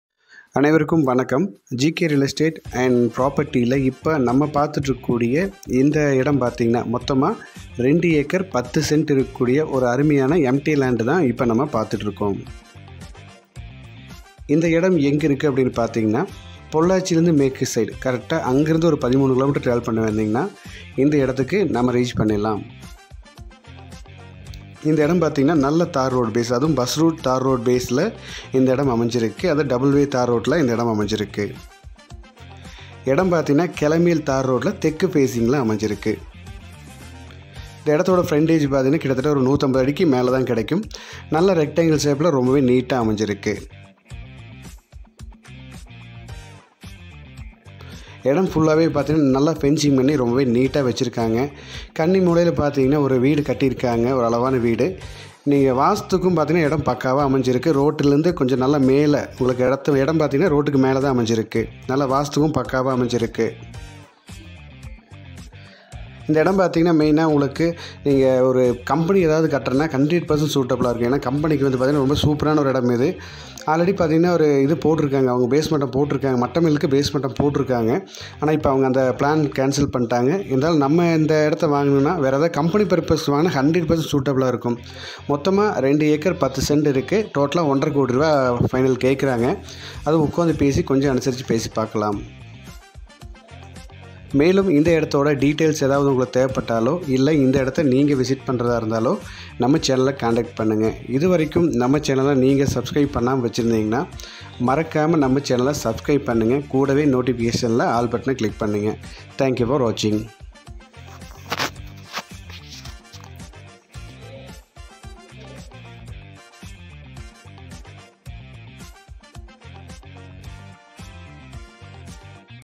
2.10 acres emptyland for sales in pollachi
2.10 acres emptyland
bus route thar road base
13 km pollachi
good investment property